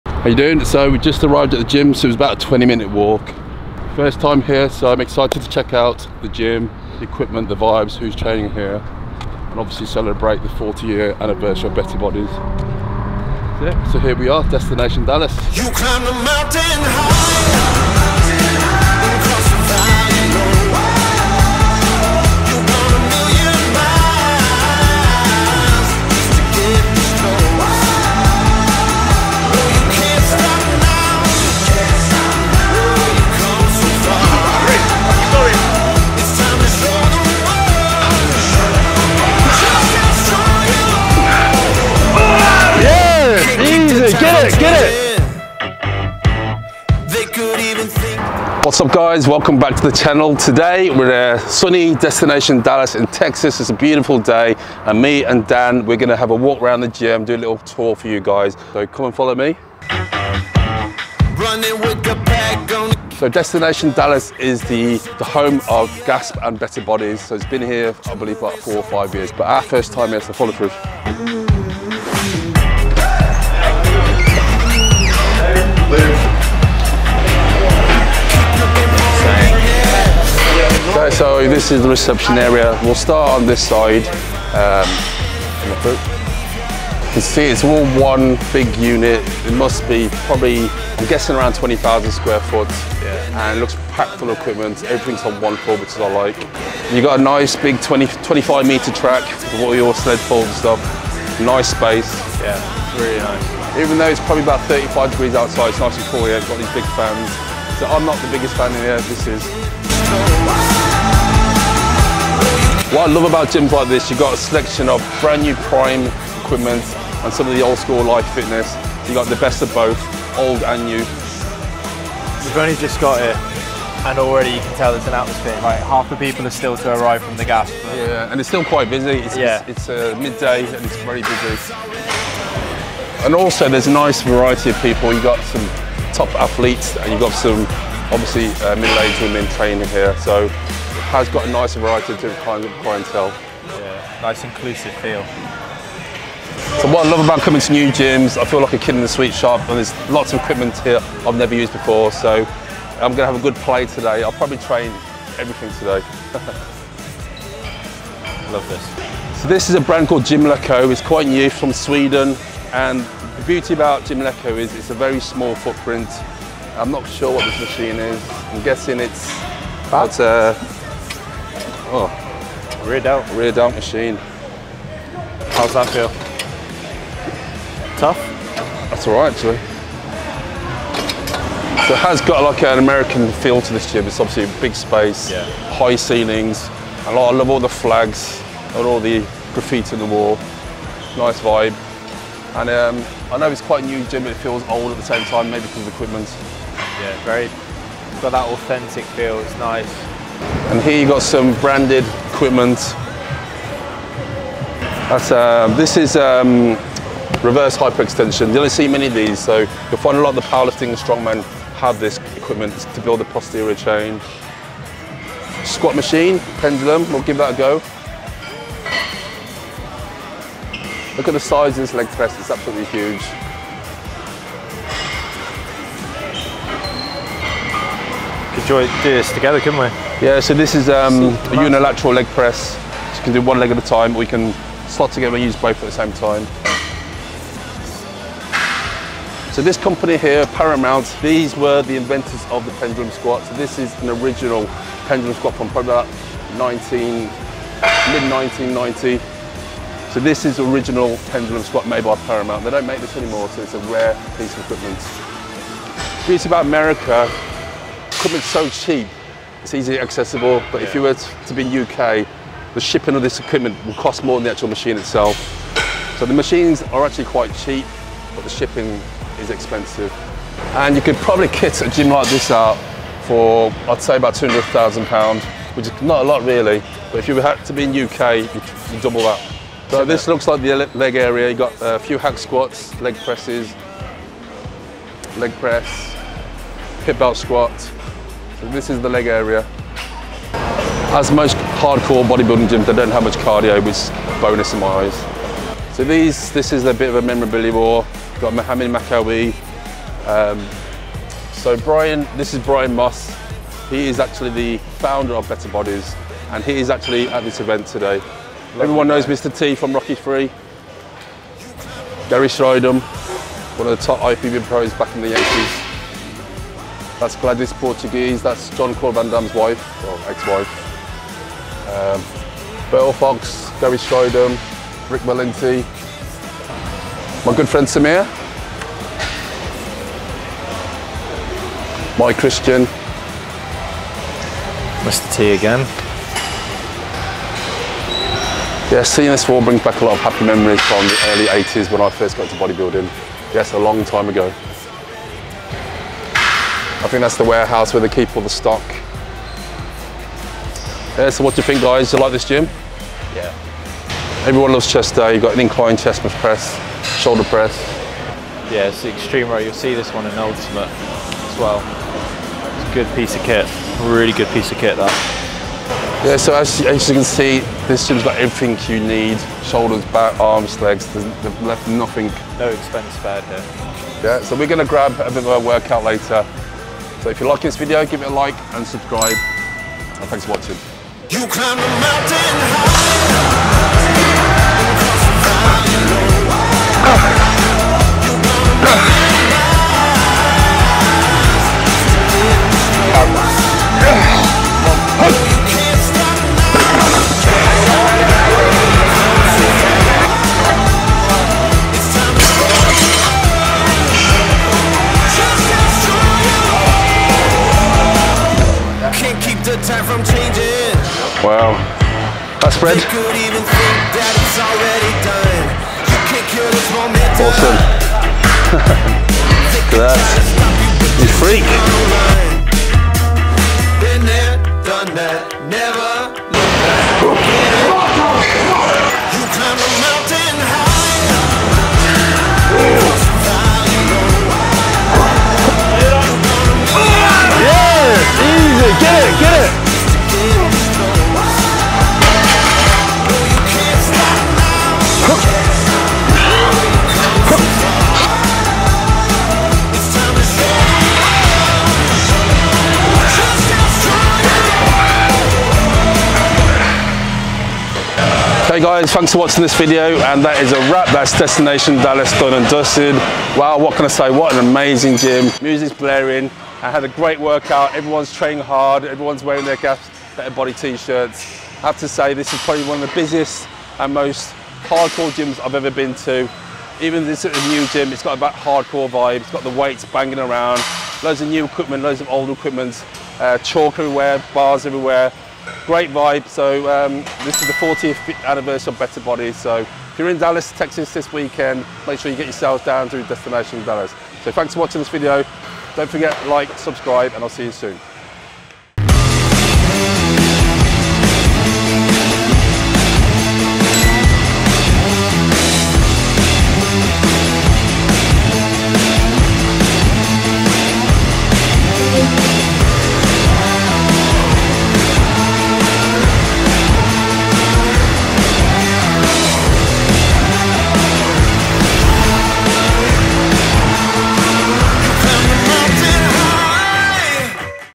Destination Dallas Gym Tour 2022 | Better Bodies 40th Year Anniversary week
Today's video takes us to Destination Dallas in Texas home of Gasp and Better Bodies Gym and we came here to celebrate the 40th year anniversary of Better Bodies clothing.

This gym is packed with some of the best equipment you can find and holds a great combination of new and old school equipment.

A definite must gym visit if you love great hardcore bodybuilding gyms!

Sony A6400
Rodeo Mic
Ronin SC Gimble
ComicaX-D D2 Boom wireless mic